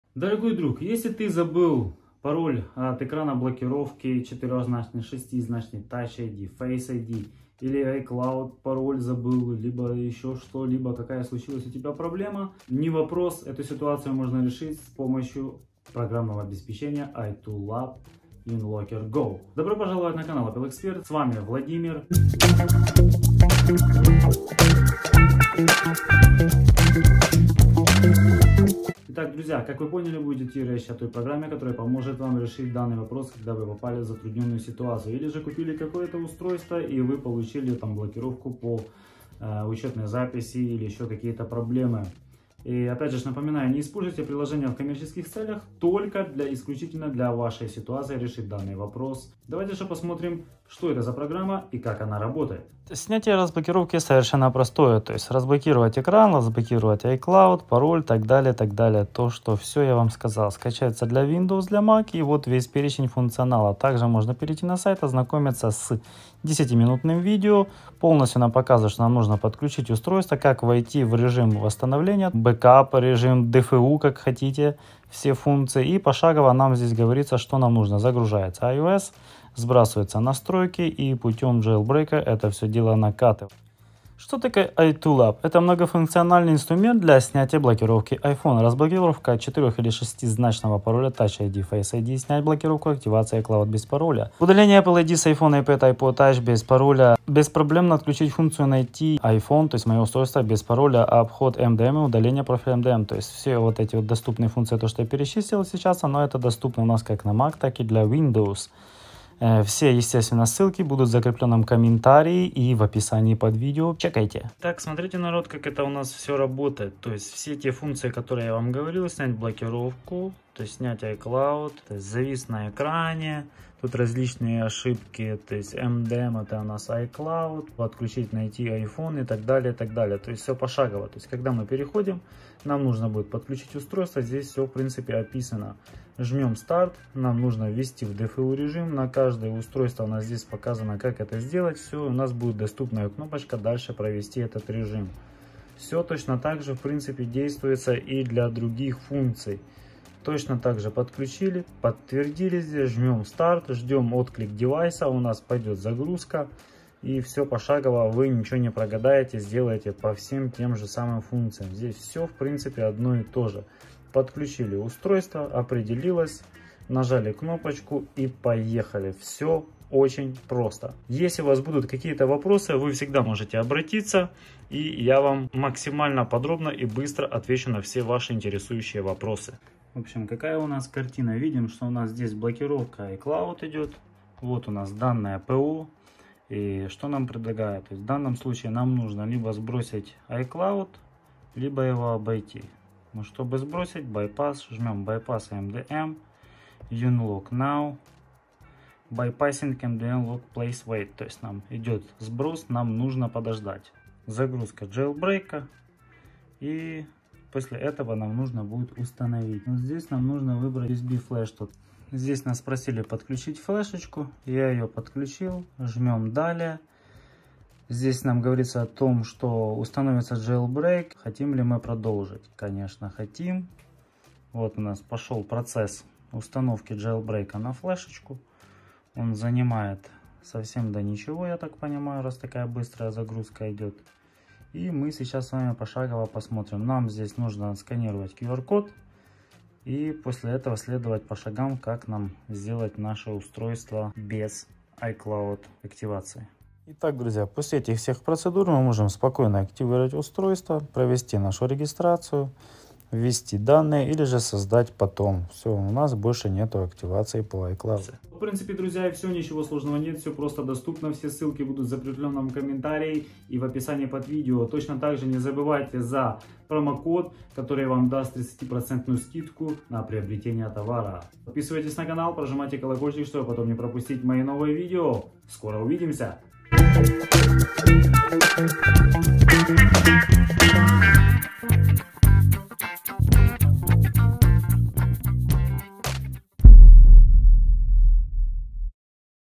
Как снятие блокировки активации iPhone/iPad 5 способов (2021)
iToolab UnlockGO - это универсальный инструмент для разблокировки, который помогает снимать блокировки экрана, блокировки активации iCloud, Apple iD и найти iPhone без учетных данных. Это один из самых простых и быстрых способов войти в заблокированное устройство Apple!

Использовать ПО только для решения своей ситуации не использовать в коммерческих целях!